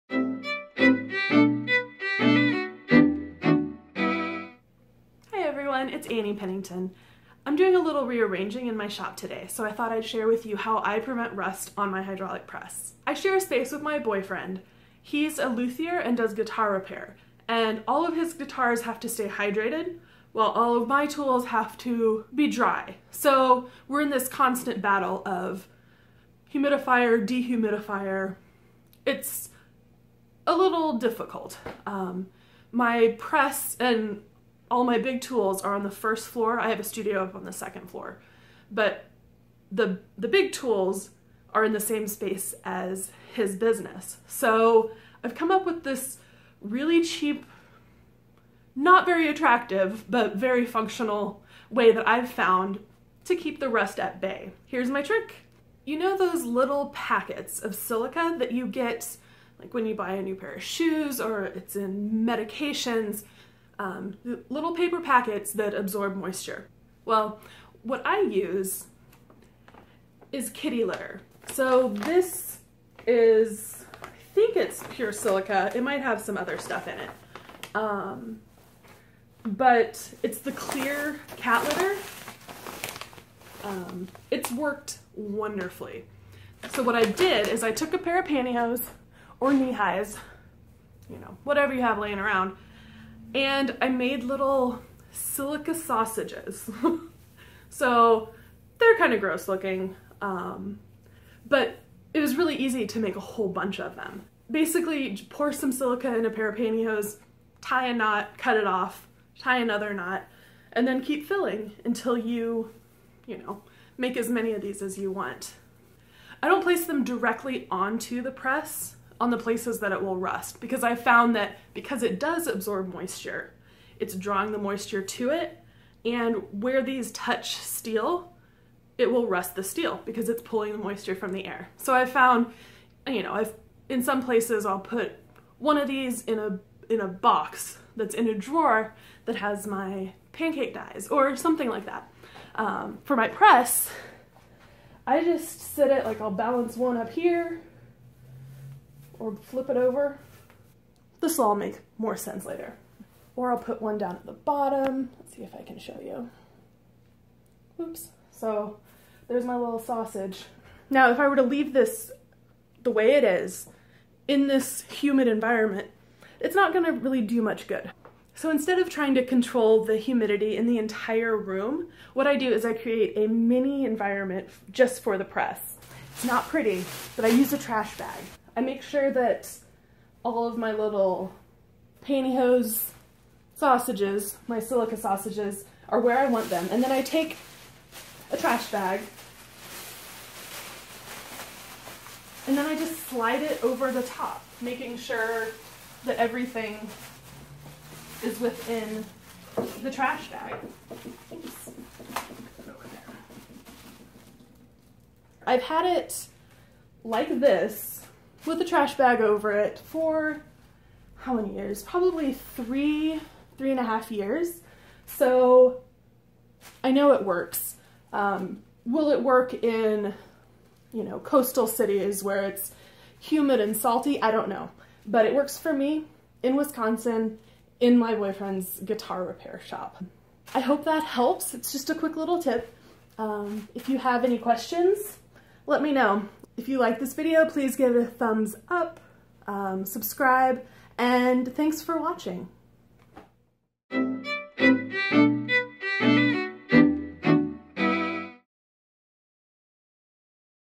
It Works! How I Prevent Rust on My Hydraulic Press in a Humid Environment
I'm in a constant battle with humidity to prevent my tools from rusting, and I found a solution. My shop is in my boyfriend's guitar-repair business, and if you know anything about guitars, they need to stay hydrated. Not so good for my tools. See how I use kitty litter to keep my hydraulic press rust-free!